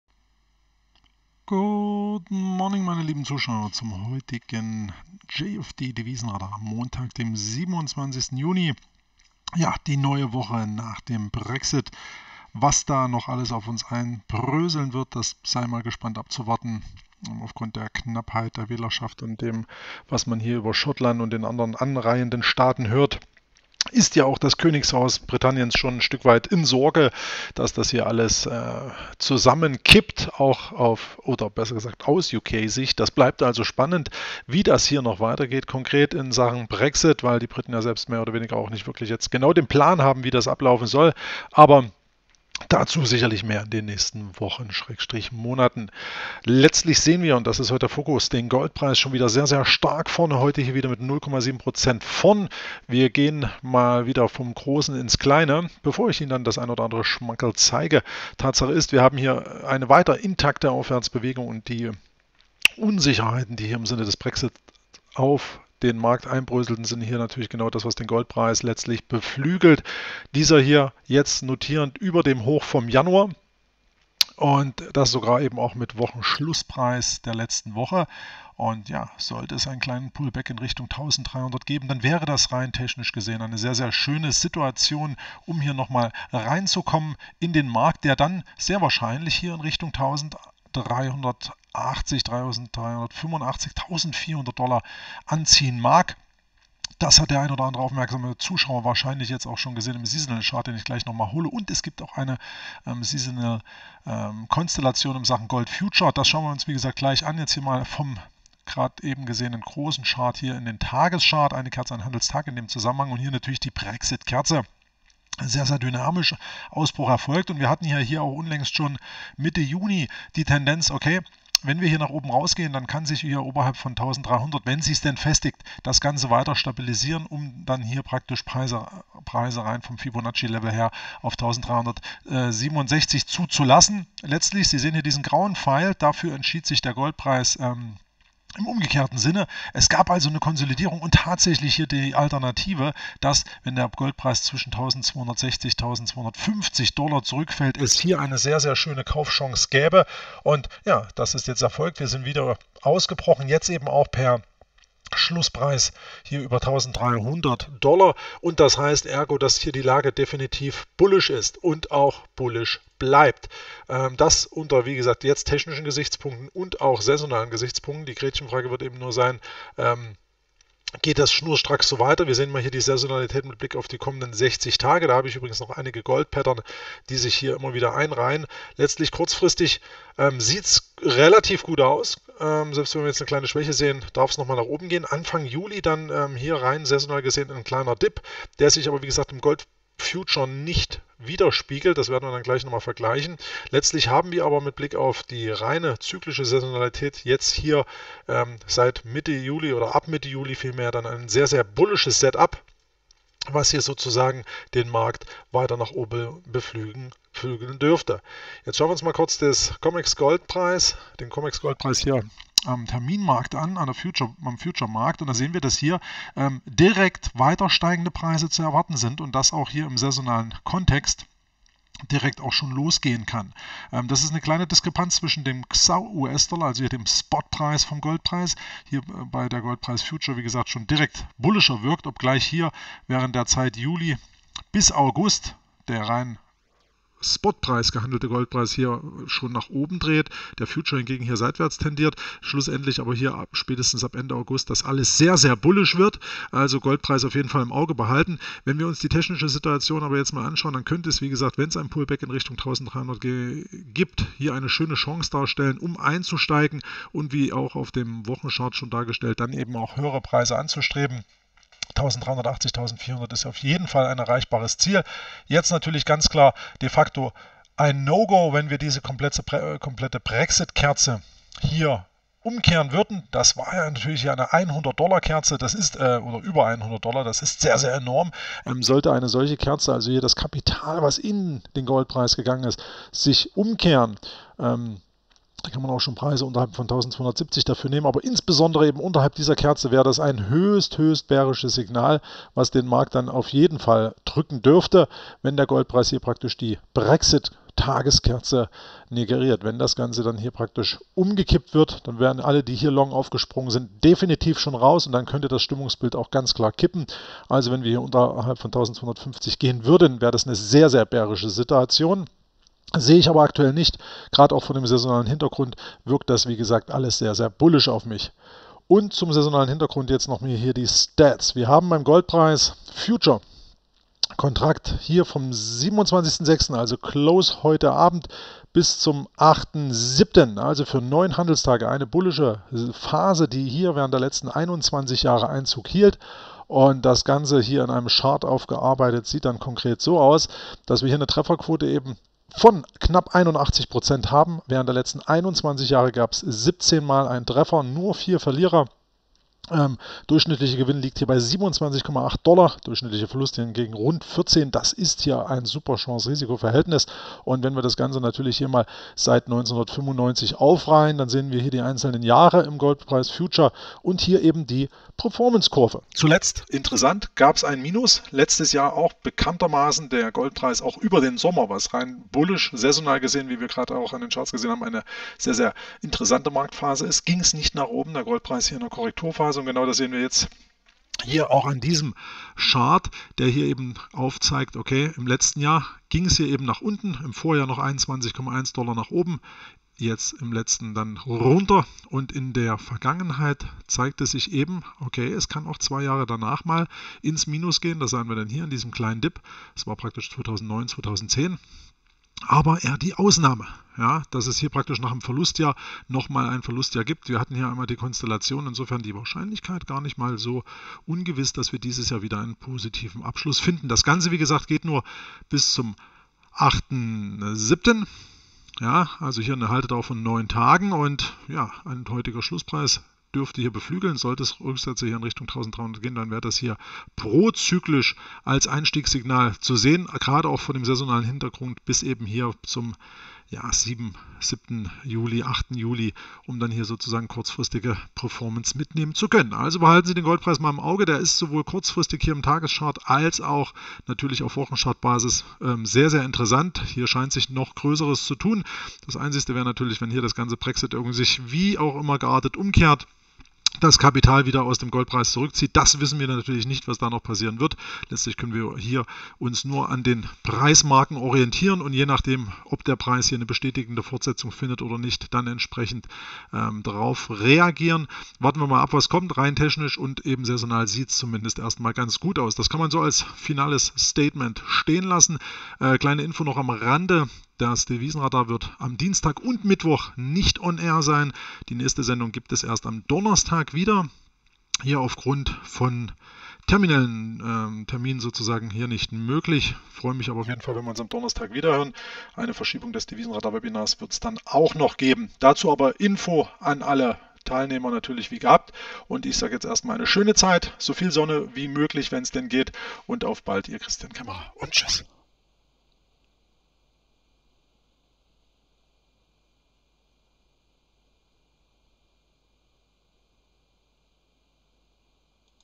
JFD Devisenradar: Gold glänzt zum Wochenauftakt weiter
Diese Stärke scheint mitsamt der saionalen und auch charttechnischen Verfassung während der kommenden Zeit eine sehr gute Vorraussetzung im Sinne weiter anziehender Preise.